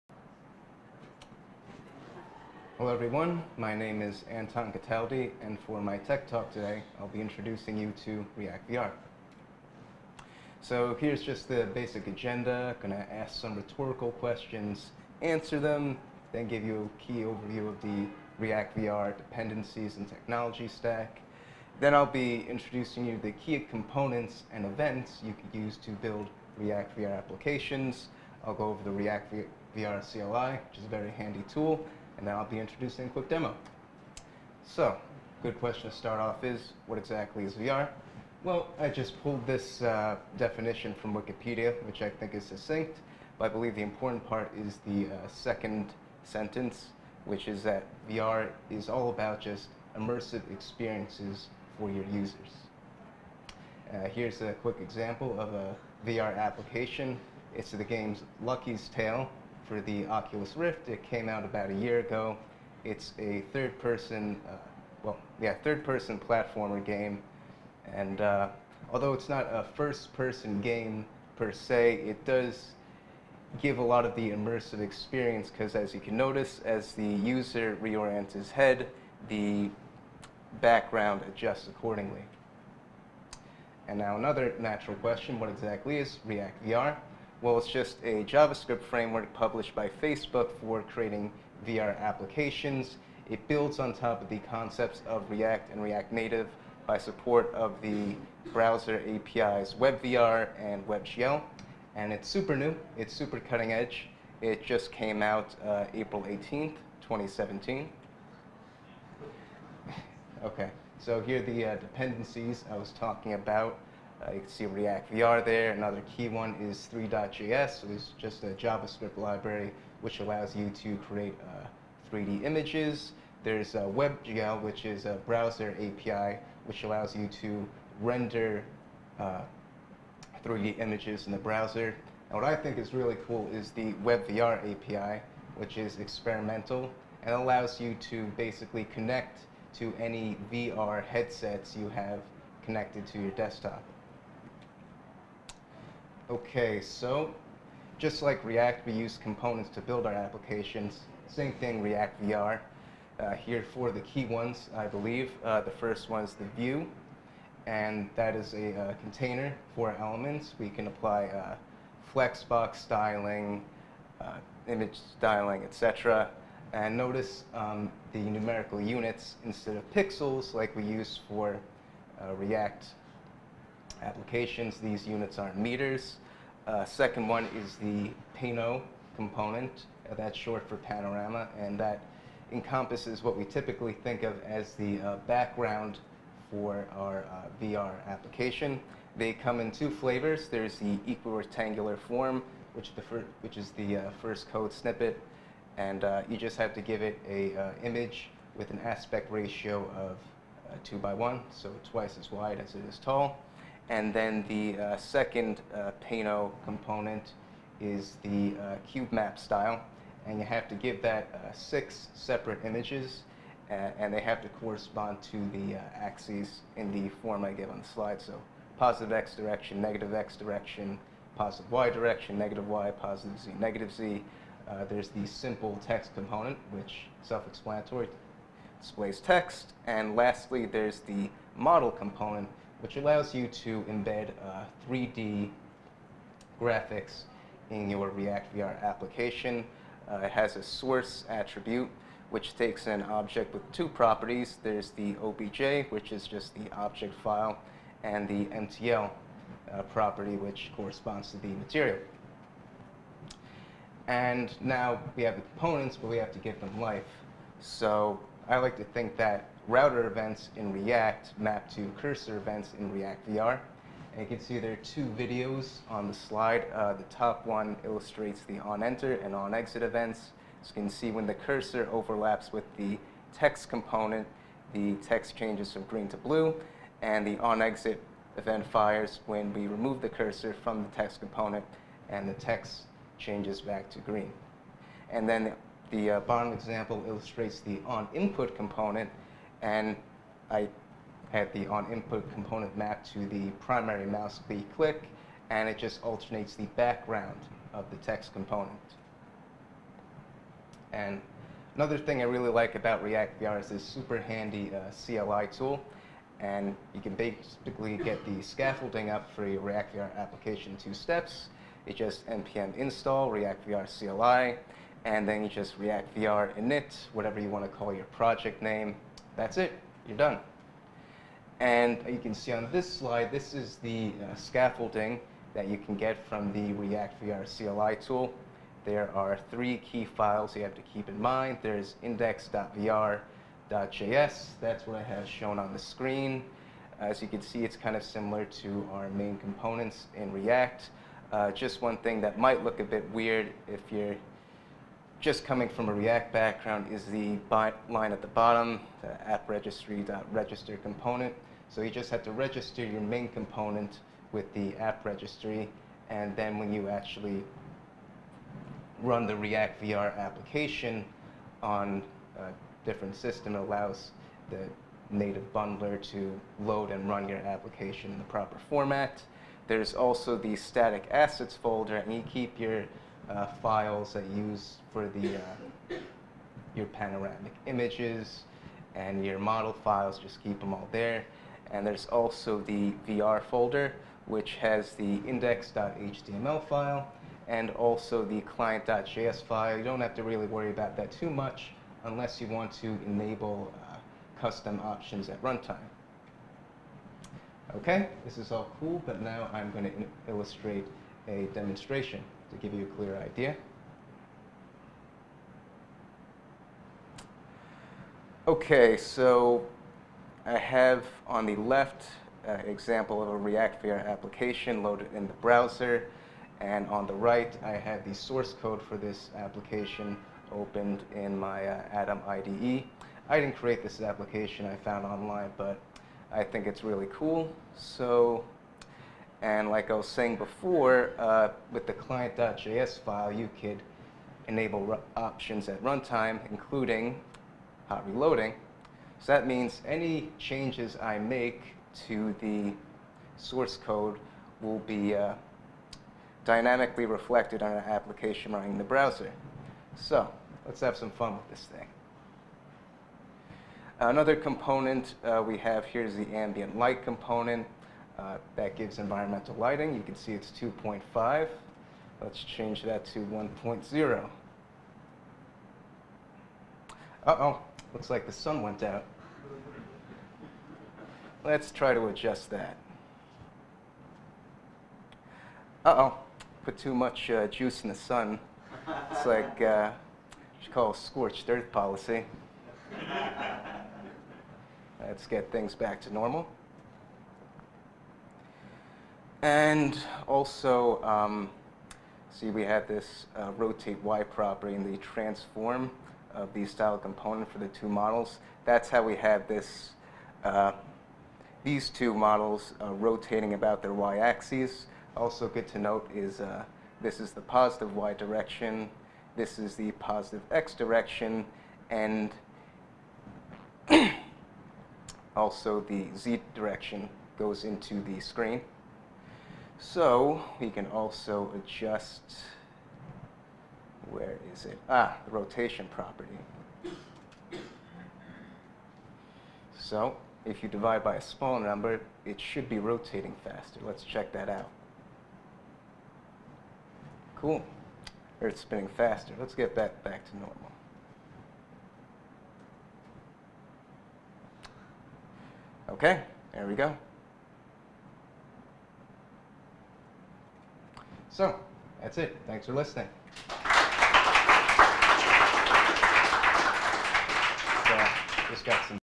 React VR Demo - An Introduction to React VR
React VR is a framework for building VR apps and interactive 360 experiences using only JavaScript. It uses the same design as React, which allows developers to compose a rich VR world and UI from declarative components. In this video, we give a brief overview of Virtual Reality concepts and introduce the React VR framework. We also talk about React VR CLI, a tool that allows developers to quickly set up the scaffolding for their applications, and showcase a demo of a React VR application.

Watch this video to learn:

- What React VR is and what are its dependencies
- Key components and events used to build React VR applications
- A demo of a React VR application